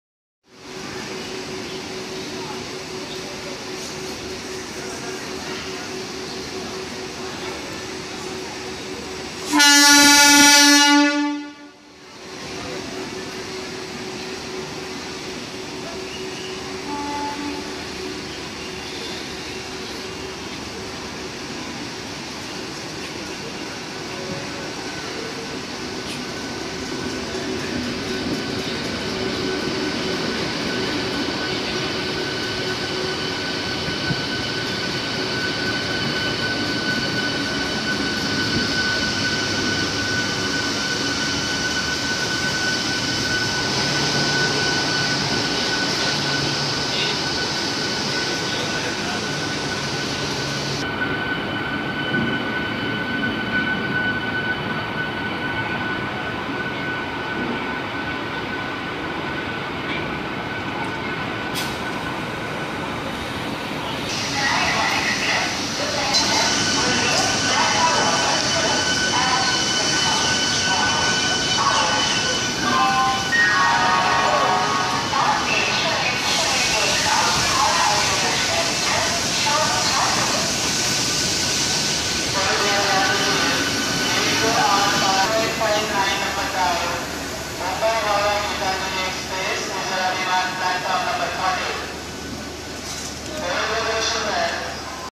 1968-2018 || 50 Years Celebration of EMU Service of SER
S.E. Rly celebrates 50 Years of EMU Service. on 1 May 1968, first EMU train in SER ran between Howrah and Panskura.
1 May 2018. to commemorate Golden Jubilee, 5th Medha Rake ran as special between Howrah and Panskura.
A Colourful programme took place in Howrah Station on 1 May 2018.